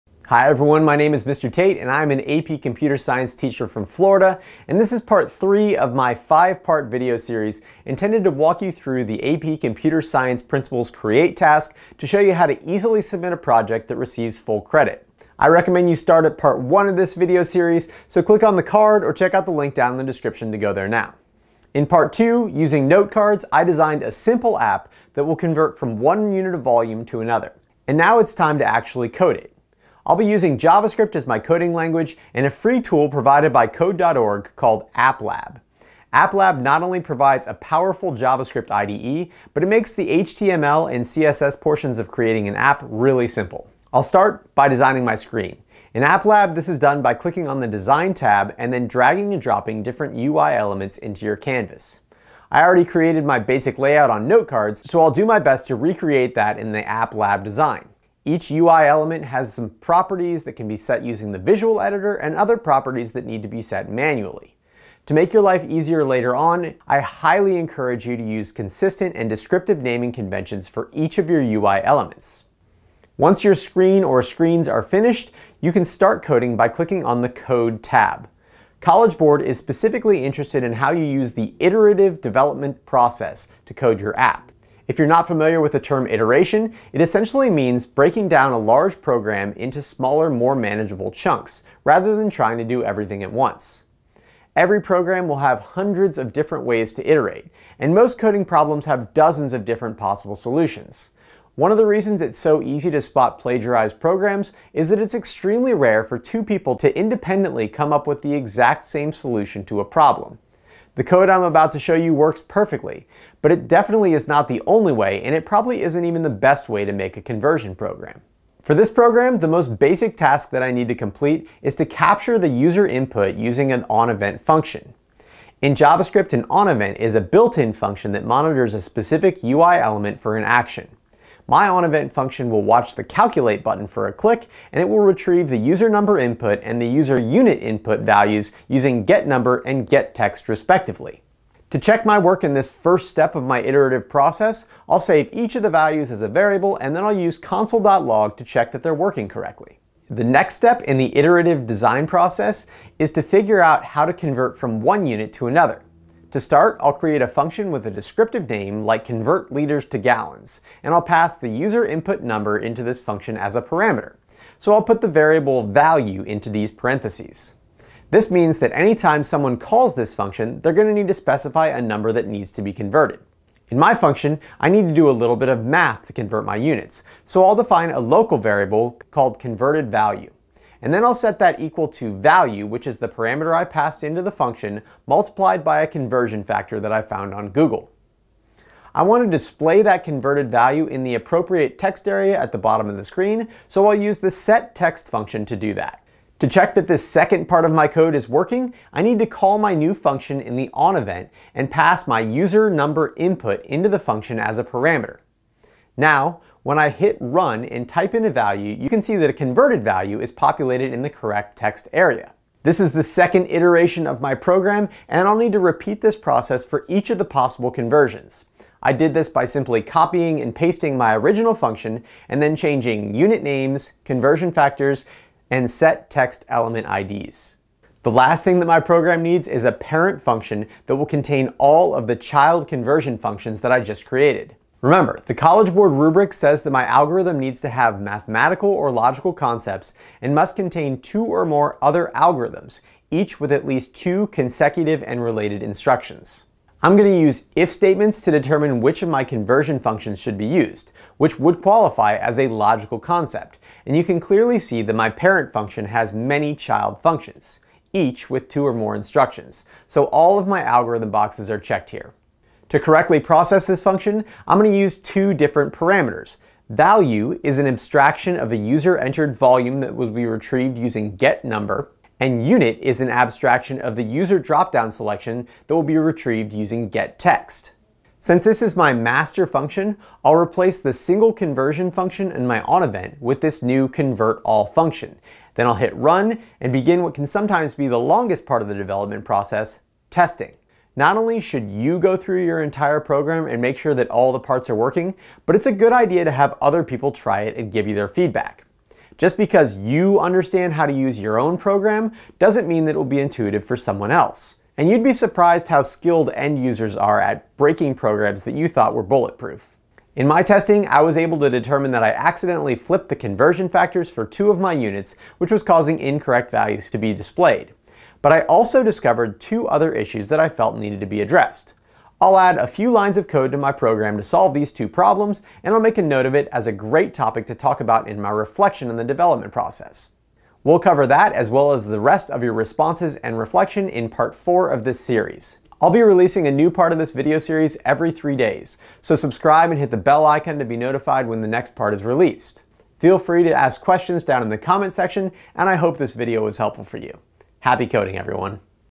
2019-2020 CREATE TASK: Part 3/5: CODING YOUR PROJECT - AP Computer Science Principles How To
IMPORTANT: While some of this video is still relevant, the create task has changed significantly in 2021.  Please see this video for a rubric walkthrough and example project

This is a How-To video for scoring 100% on the AP Computer Science Principles Create Task.  This series is divided into 5 Parts: